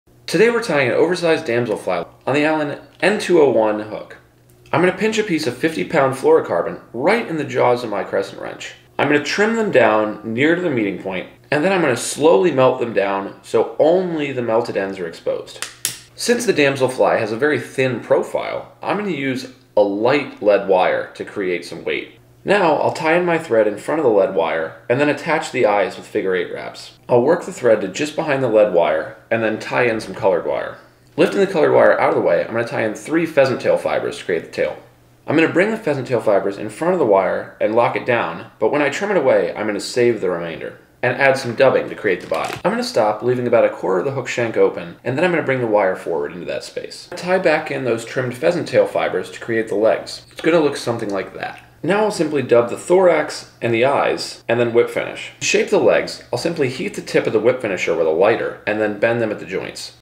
FRIDAYFLYTYING: Damsel Fly Nymph
There's a lot of ways to tie a damsel fly nymph imitation, but this version is a good realistic match, plus it incorporates a couple neat techniques for creating eyes and legs that you'll want to add to your fly tying repertoire. We've tied the pattern a bit oversize here to make it easier to follow! Just don't burn yourself while making the eggs and shaping the legs!
- Hook: N201 -
- Weighted Wire -
- Colored Wire -
- Pheasant Tail Fibers -
- Dubbing -
- Fluorocarbon for eyes (30-50lb) -